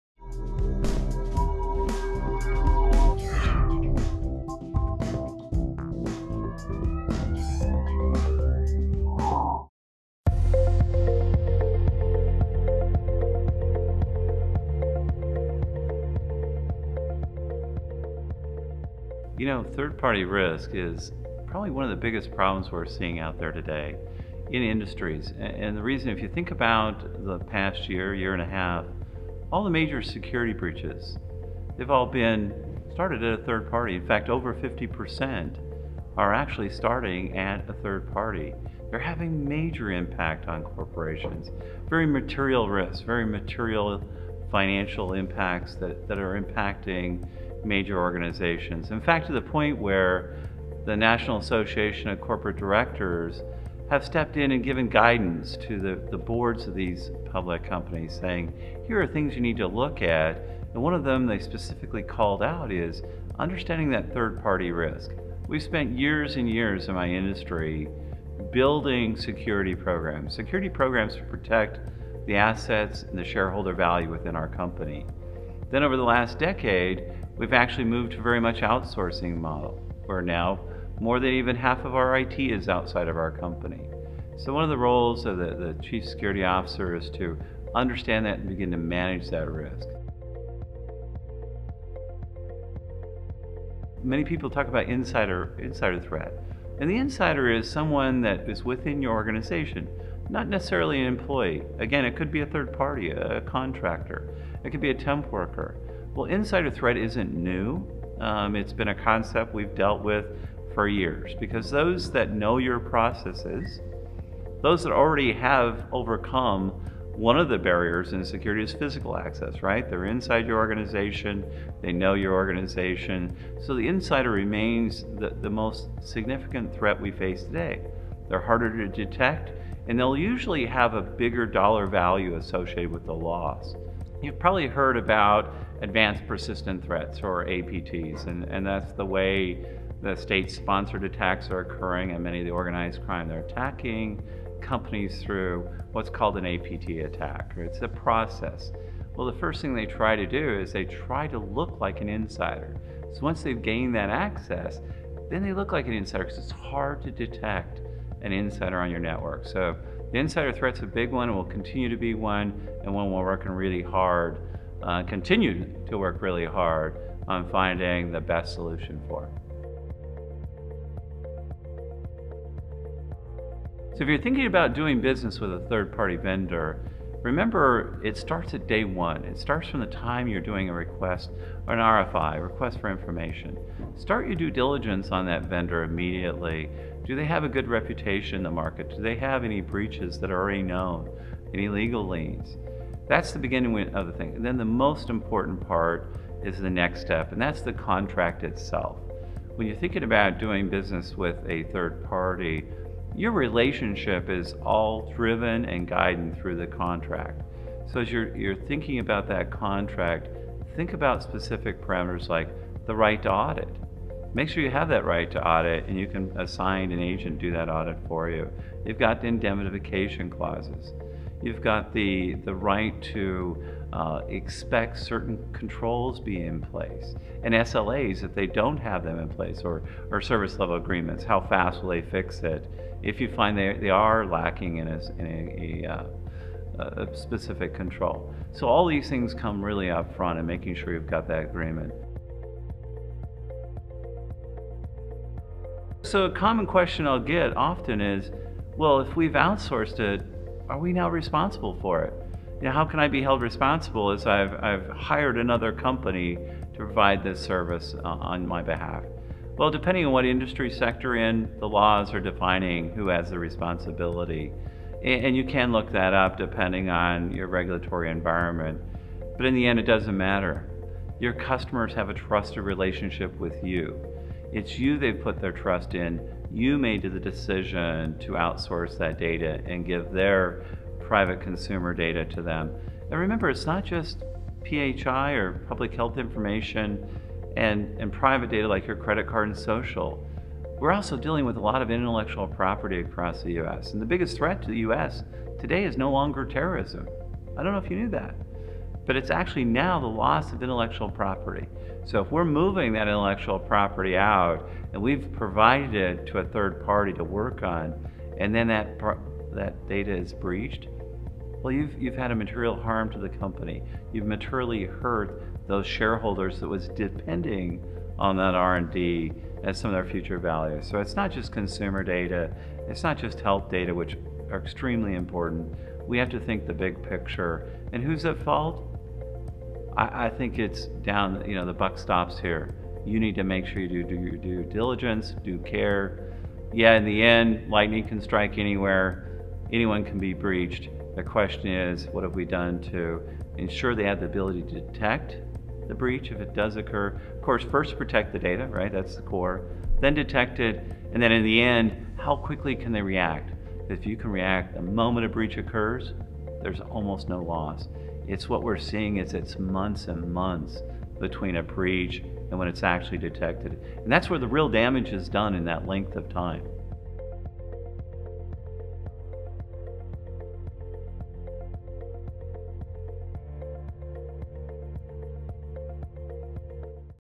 Data Security and Third-Party Risk Management | In Focus
The majority of breaches occur as a result of third parties. In this interview, James Christiansen, VP of information risk management at Optiv Security, discusses third-party risk with GSCPA In Focus and provides insight on data security, managing risk, and data breach liability.